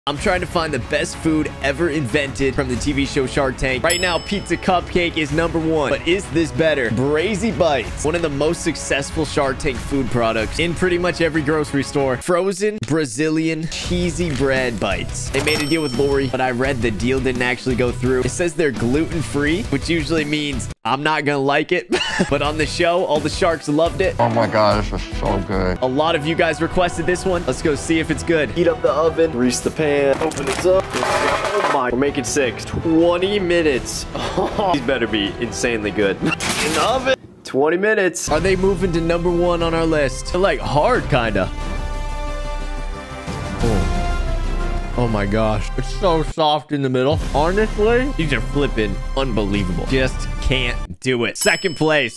Part 5 tasting & rating every food from the TV show Shark Tank...best frozen snack ever?
This is the Brazi Bites from Shark Tank review. I am reviewing every food product from Shark Tank to find the best Shark Tank pitch ever. Join my journey as I rank the best and worst Shark Tank food products and ideas. This should be a fun product review series. YouTube is awesome! We love Shark Tank. Episode 5: Brazi Bites review. Episode 1 in my other series was the Scrub Daddy sponge from Shark Tank!